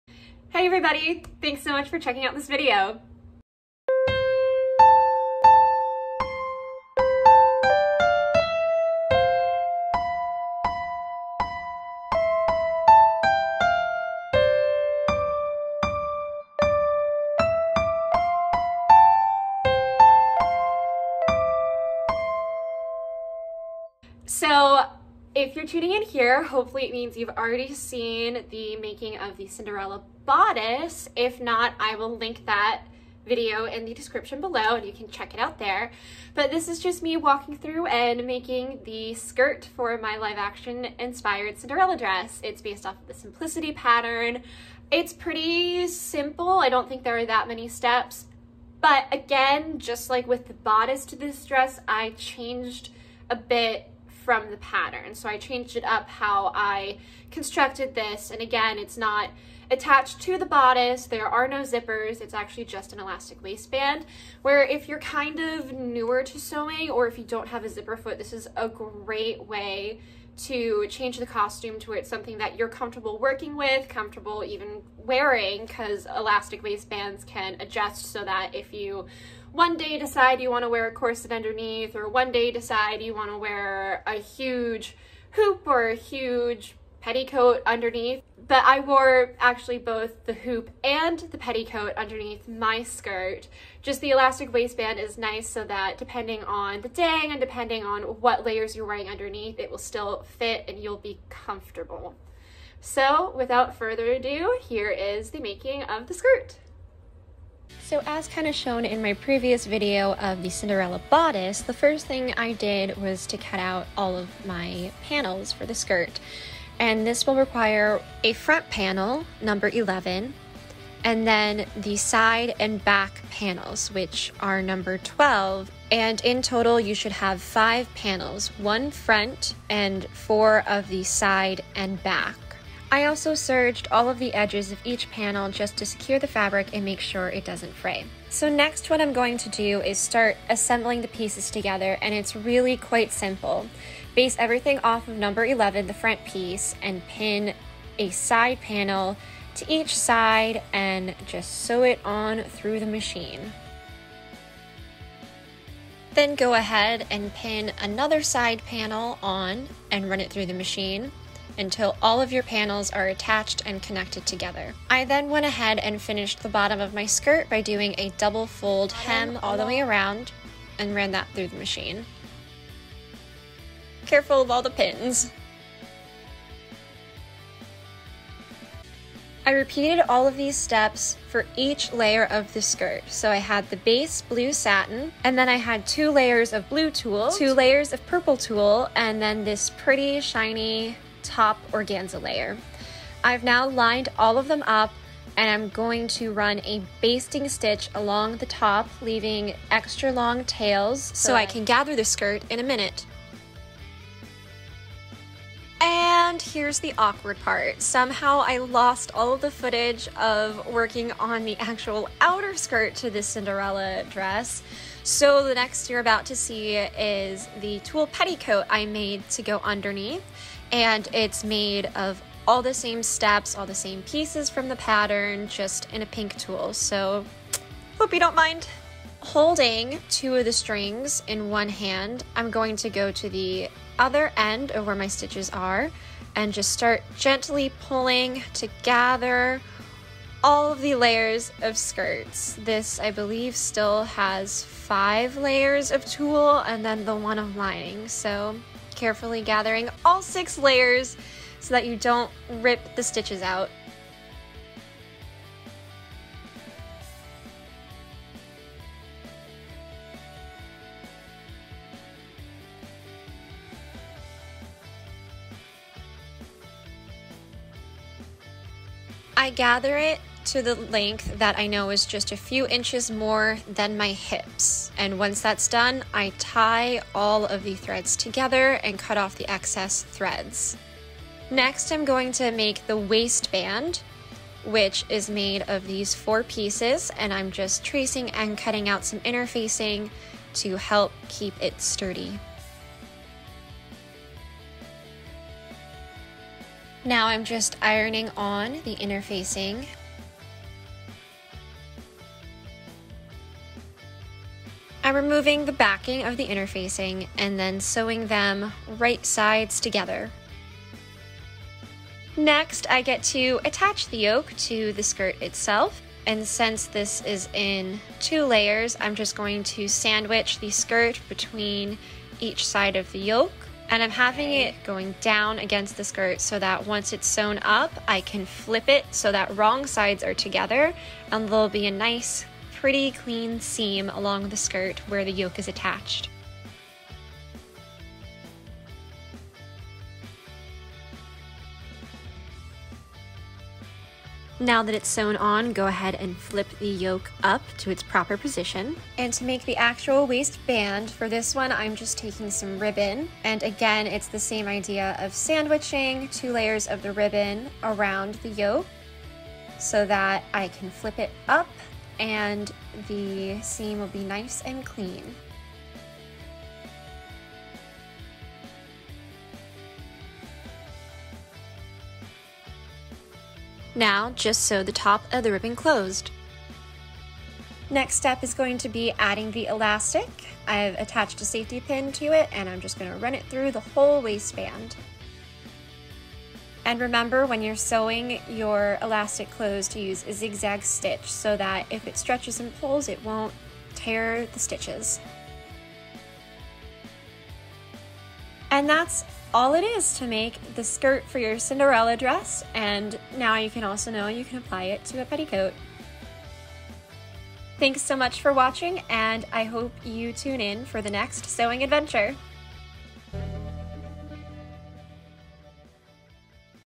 Making Simplicity 1026 Live Action Cinderella Skirt and Bonus Petticoat (Sort Of)
Thanks for checking out this video! Here is Part II of Making Simplicity 1026 Live Action Cinderella (Sort Of).

Once again, I've changed the pattern and steps of assembly a little bit so that I can make this costume work best for me. I also ended up making a petticoat with this same pattern. So that is included in this video as well!